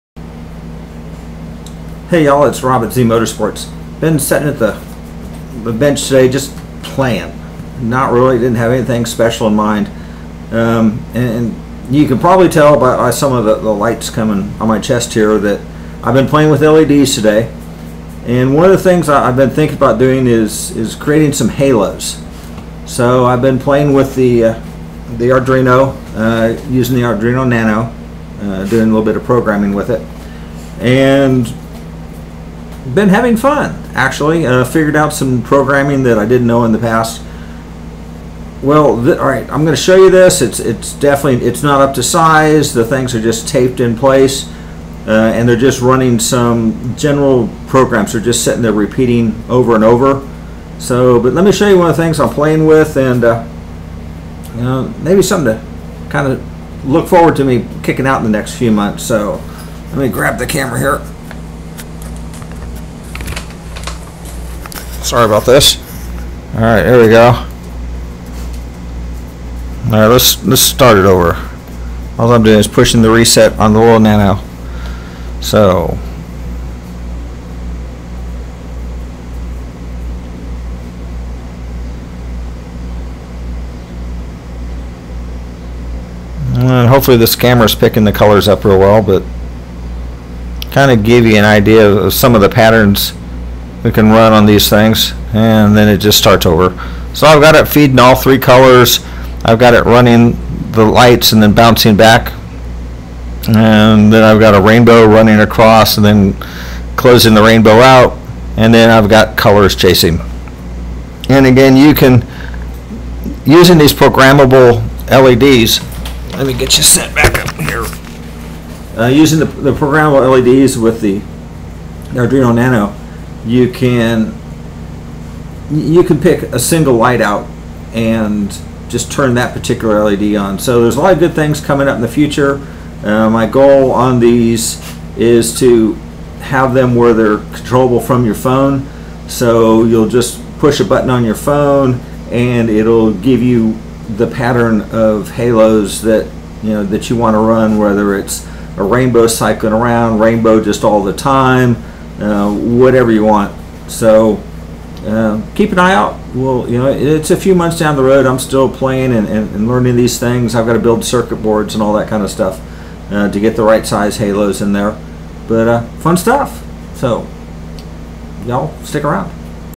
Arduino Neopixles Halo beginning
Starting the learning curve on programming Neopixel Rings for Halos on my Eclipse. Here are some test programs I came up with while learning the possibilities.